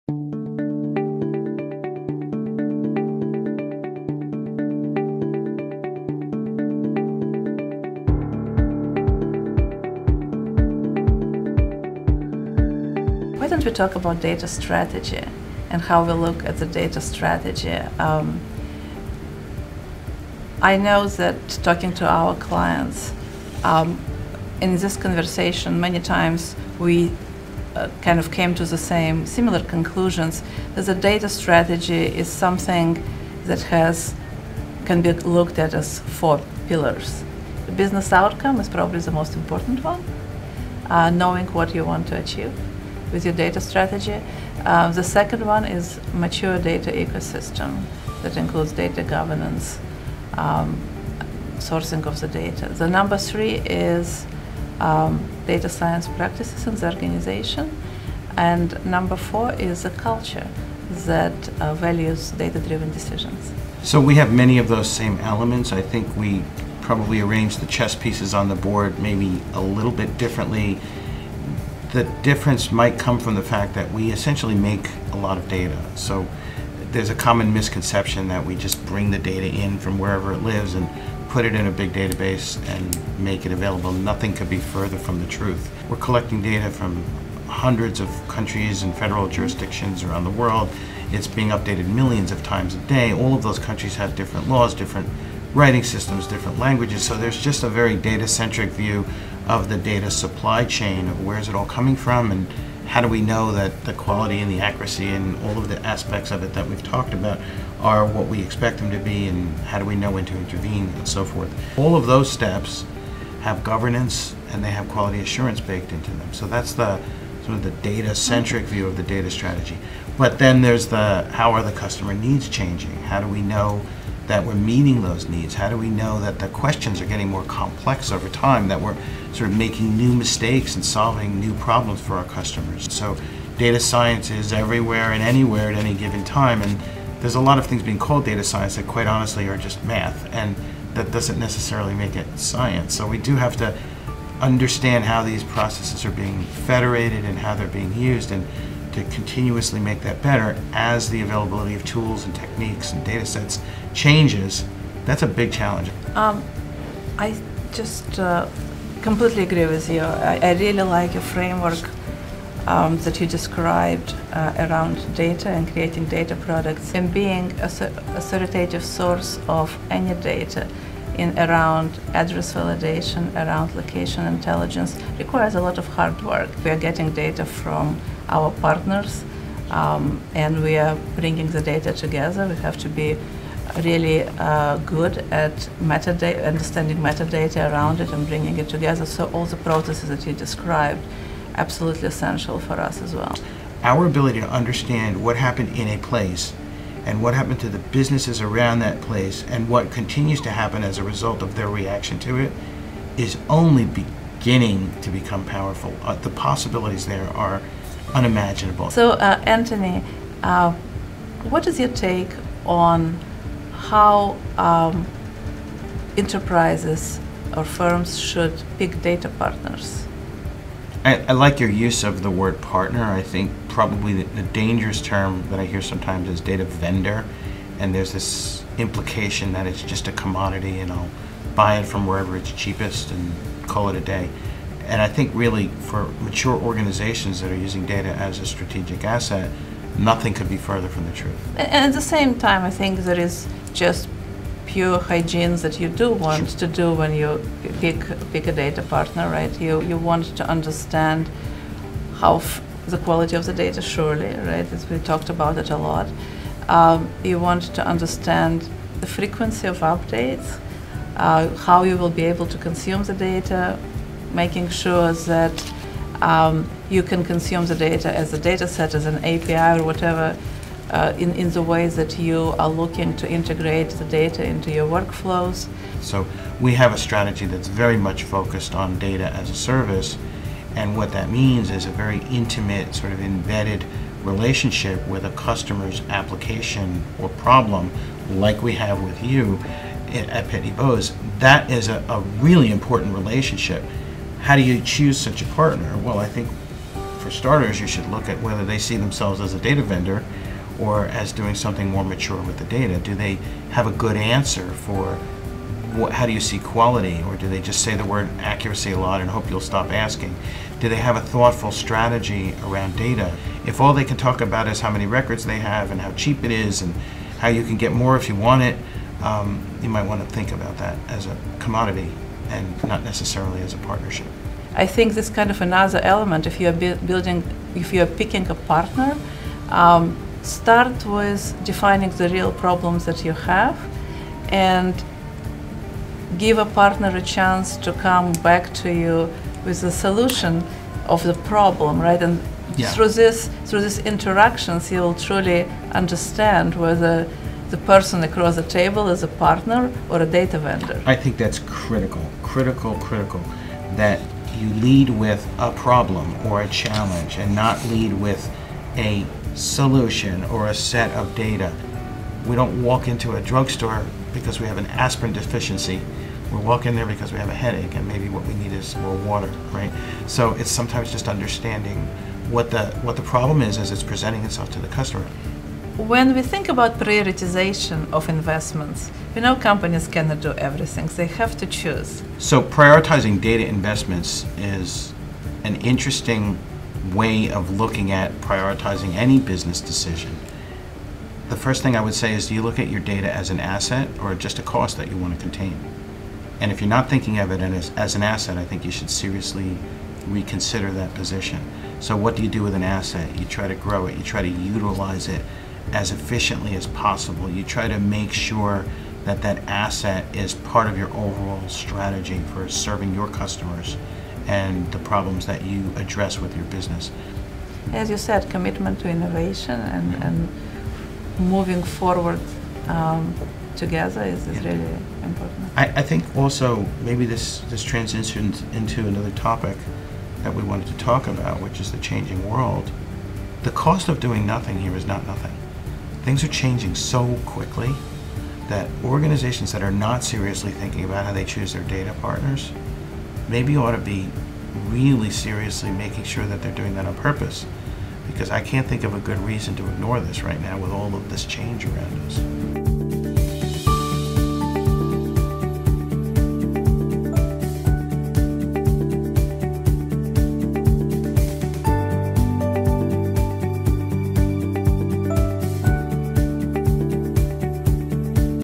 Data Strategy - Key Pillars That Define Success
Sit in on a conversation about defining successful data collection strategies with Dun & Bradstreet's Chief Data Scientist, Anthony Scriffignano, and Olga Lagunova, Pitney Bowes' Chief Data Analytics Officer. Hear how expensive the cost of doing nothing with your data solutions really is.

 This conversation can help guide data management solutions and will provide insights into the rapidly evolving master data industry. Hear how new data-as-a-service solutions are providing IOT data and connected data tools that will help accelerate your business' performance.